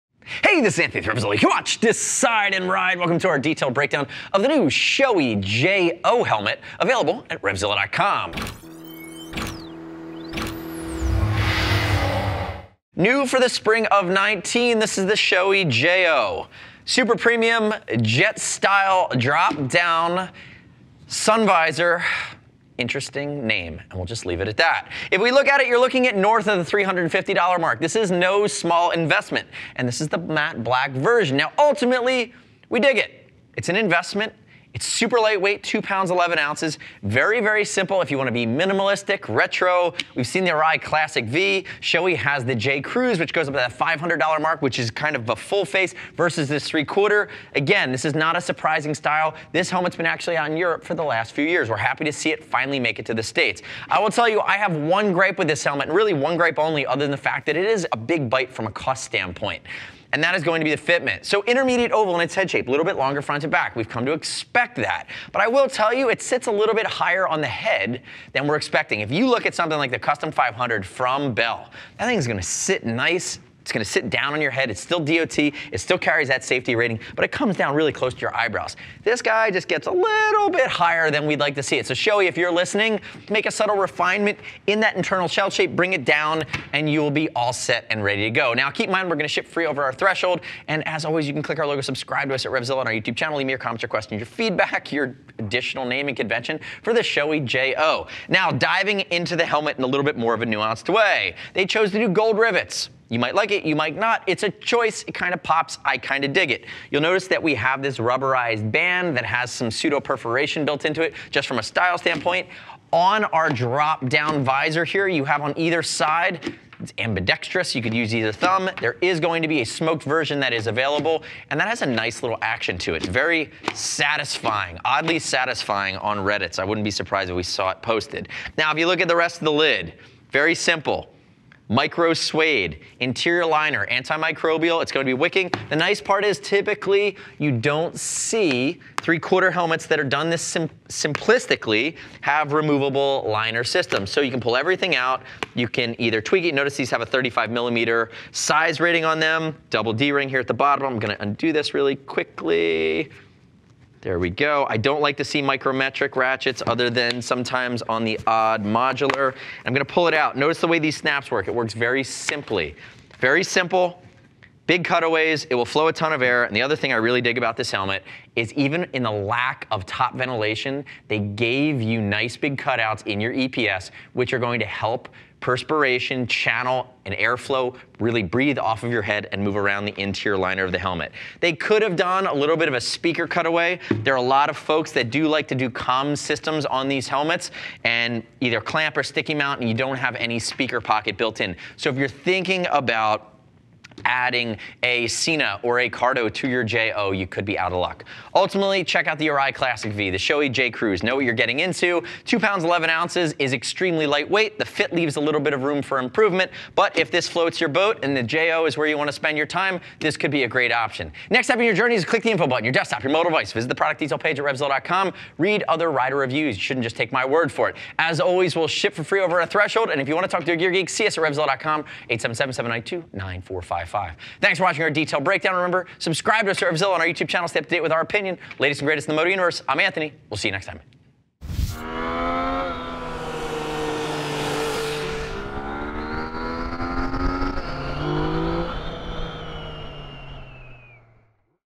Shoei J.O. Helmet Review
OK, let’s get something straight. This sweet new retro helmet from Schoei is pronounced "jay-oh" not "joe". The J.O. is crafted with an AIM (Advanced Integrated Matrix) shell in three sizes for a sleek retro look for multiple head sizes. A three-position drop down CJ-3 sun visor keeps the sun at bay while you ride. Additional features include nubuck leather trim, eyeglass-compatible cheek pads, and a removable interior that can be washed. The Shoei J.O. helmet is DOT approved.